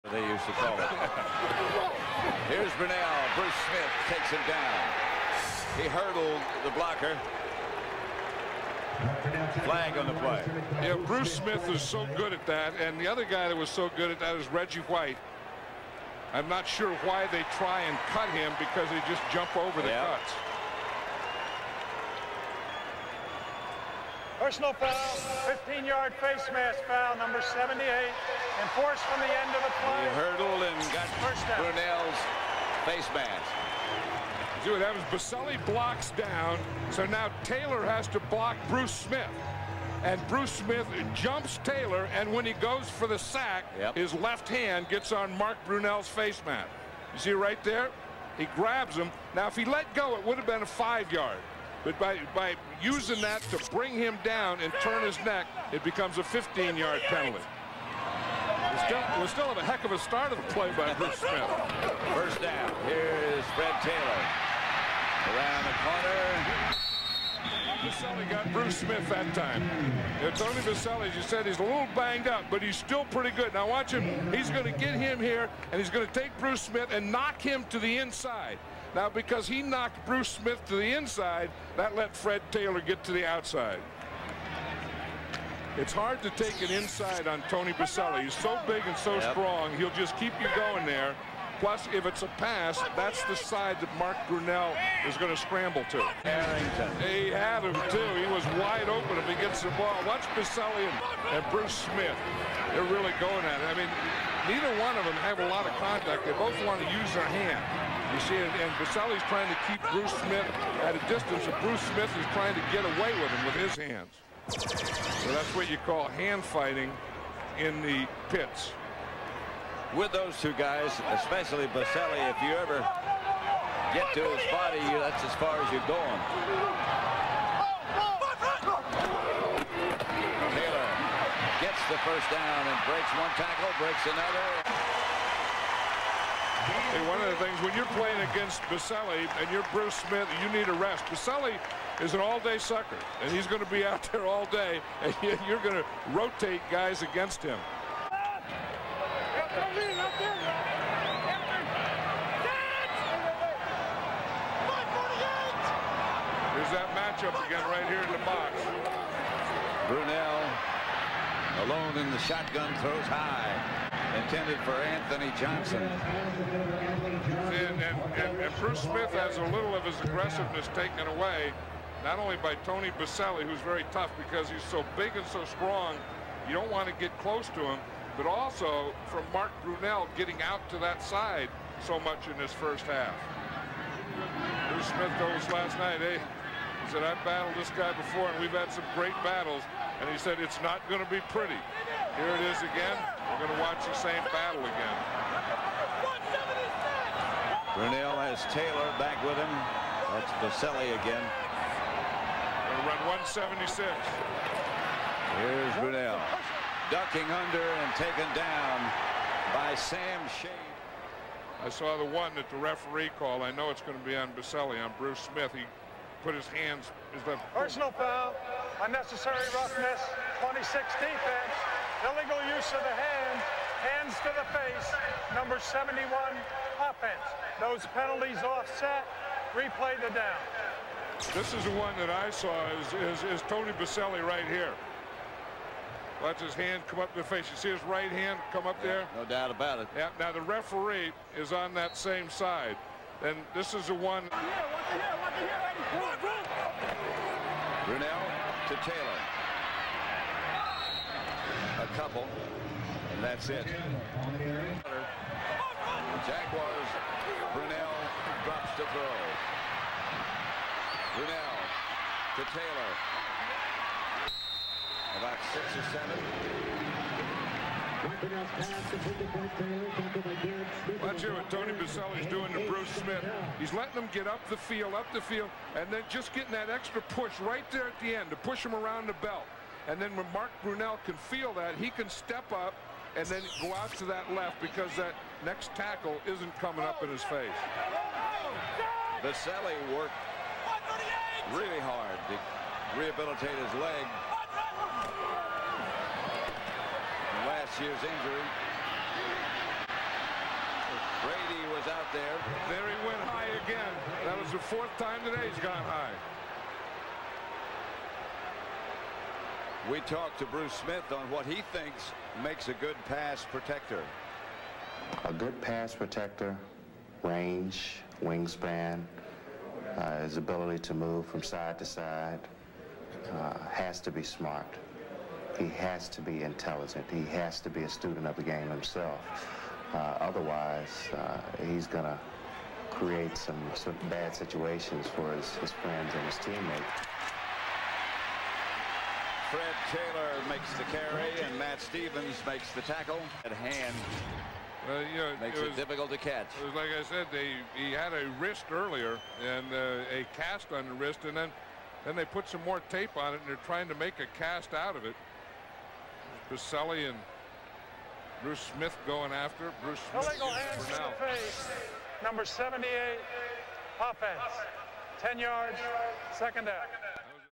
Tony Boseli vs Bruce Smith 🤬 NFL Hall of Fame Matchup! (OL vs DL) Hall of Fame Game highlights
Hall of Famer Tony Boseli & the Jacksonville Jaguars match up against Hall of Famer Bruce Smith & the Washington Redskins in the 2000 NFL season
OL vs DL Matchup